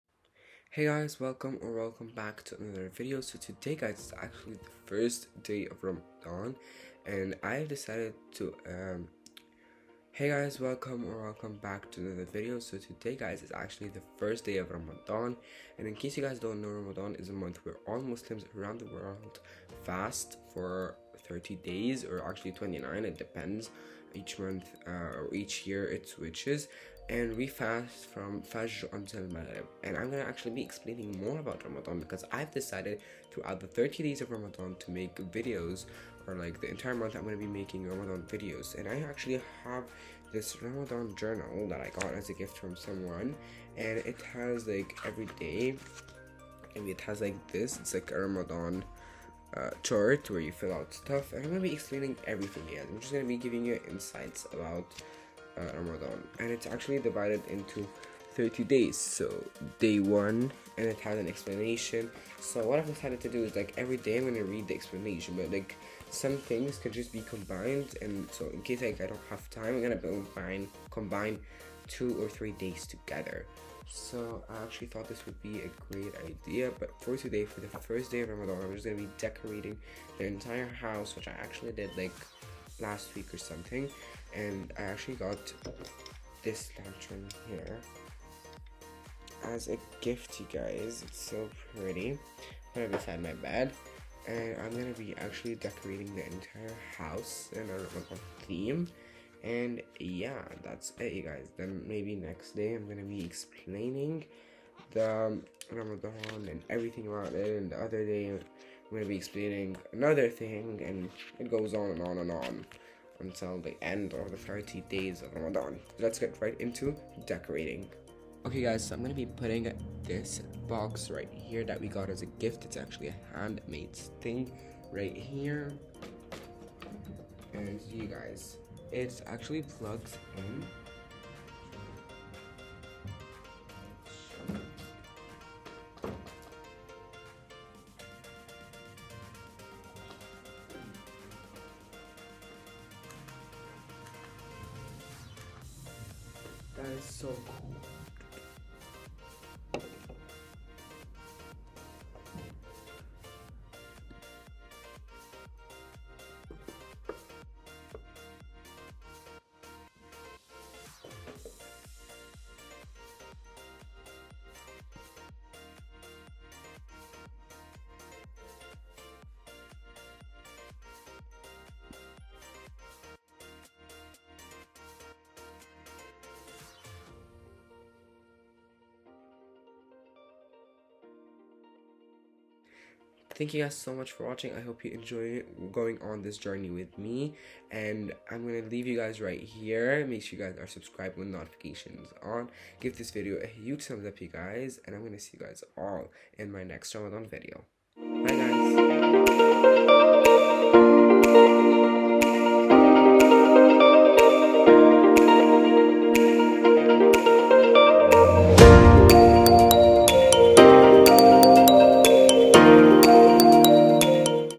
Prep with me for Ramadan | Ramadan Vlog 1
Today I'm Decorating the house for Ramadan.